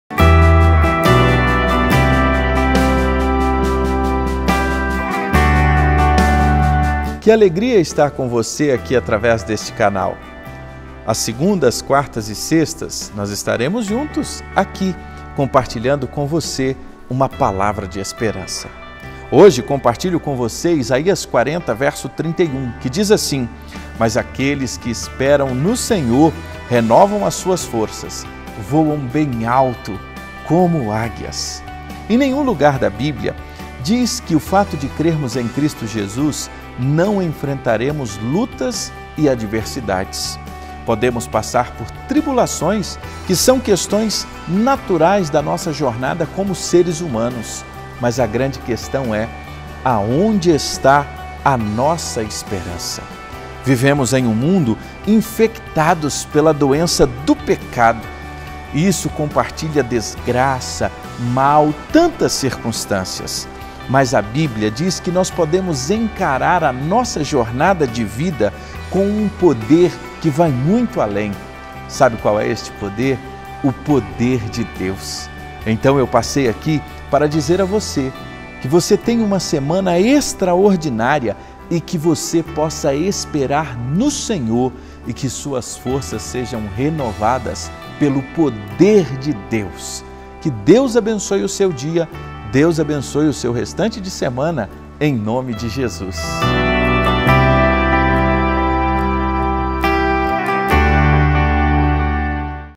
Voando como a Águia - Fabrício Freitas
PALAVRA DE ESPERANÇA | Você deseja voar como águia? Como está o seu plano de voo está semana? Quero te convidar a investir um pequeno tempo e assistir este vídeo ao iniciar este dia e seja um vencedor em nome de Jesus!